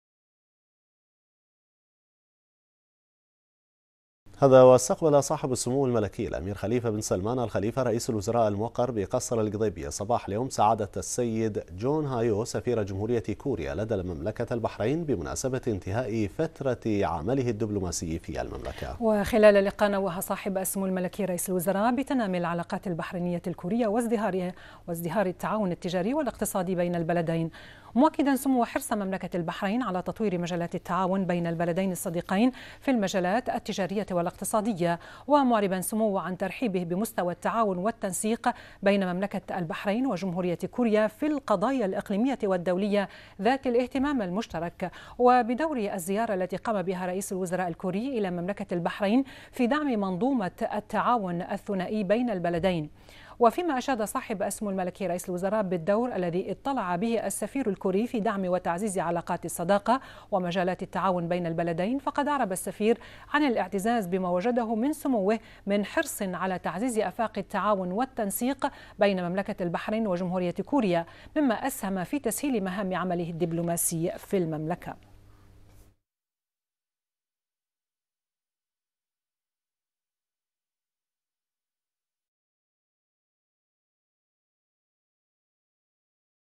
البحرين : سمو رئيس الوزراء يستقبل السفير الكوري لدى المملكة
الوصف

سمو رئيس الوزراء يستقبل السفير الكوري لدى المملكة